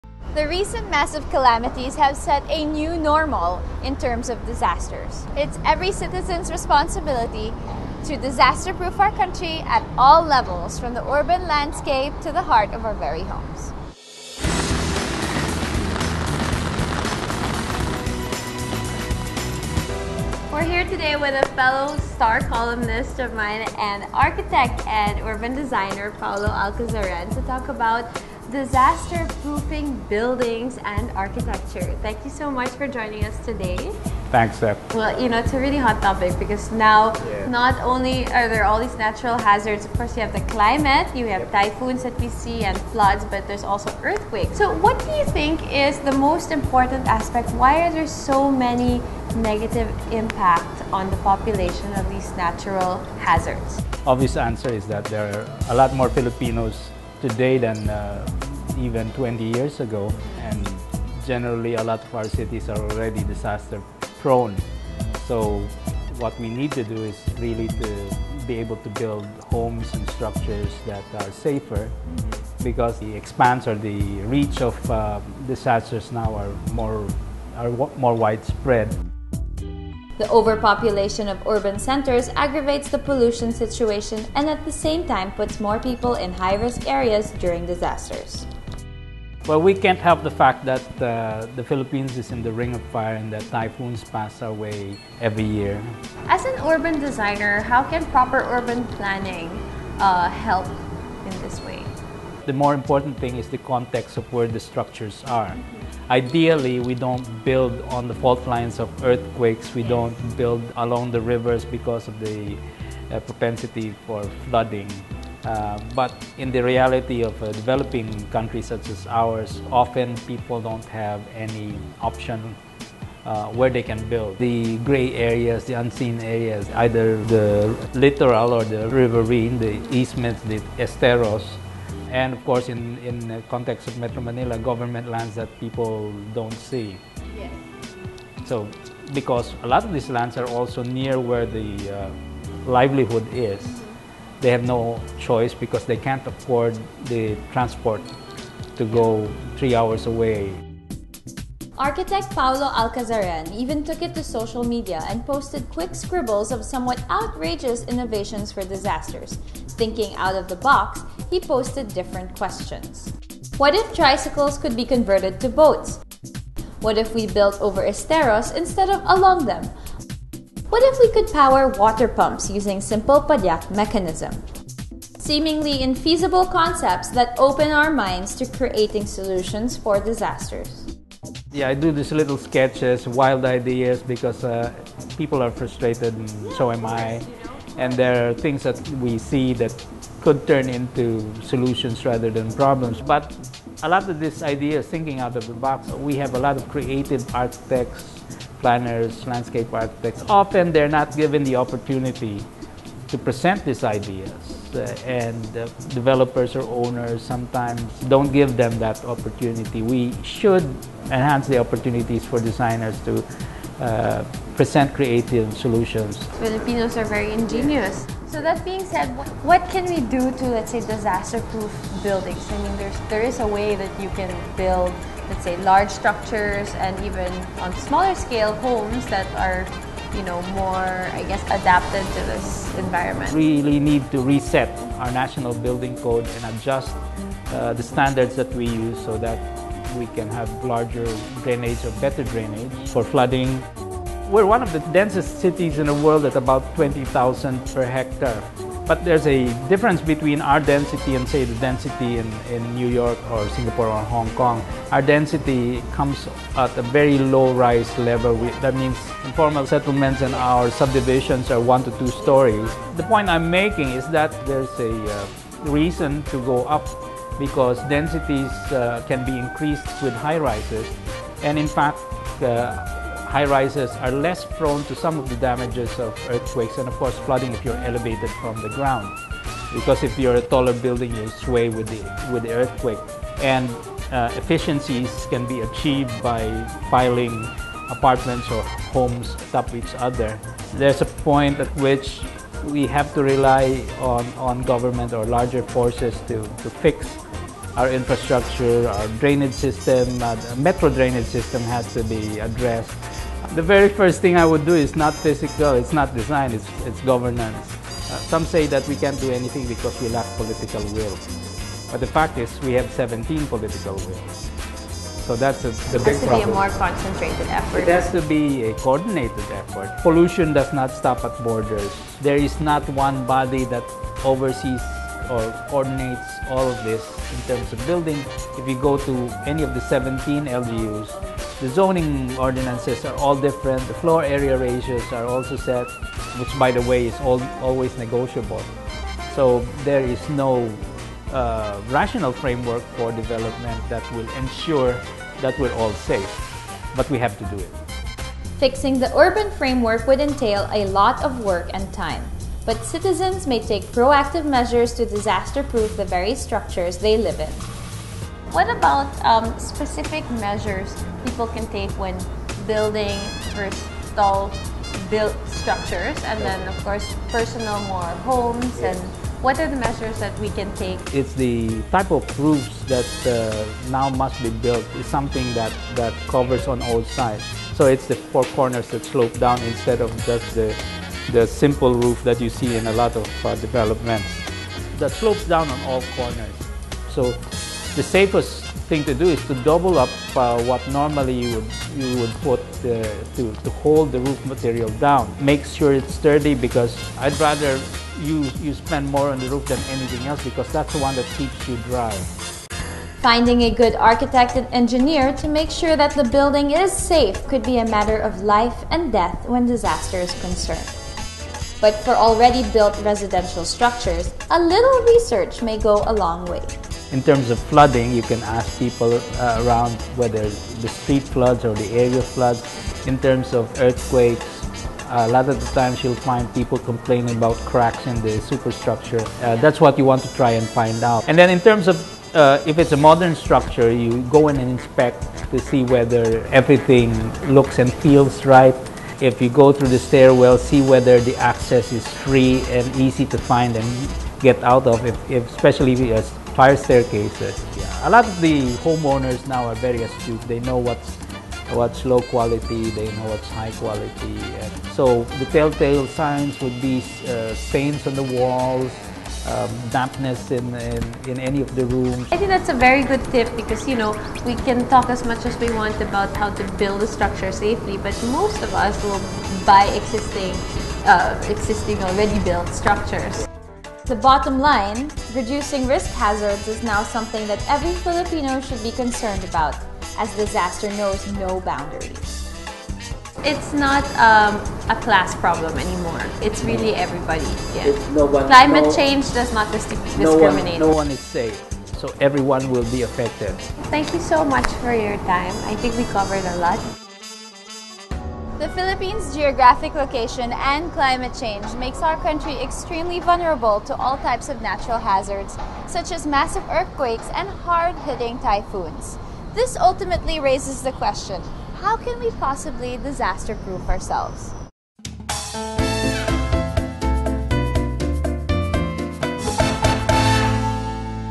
Disaster proof 101 with Architect Paulo Alcazaren
Is your home sturdy enough to protect you and your family? Stephanie sits down with Architect Paulo Alcazaren to learn about disaster-proofing a home.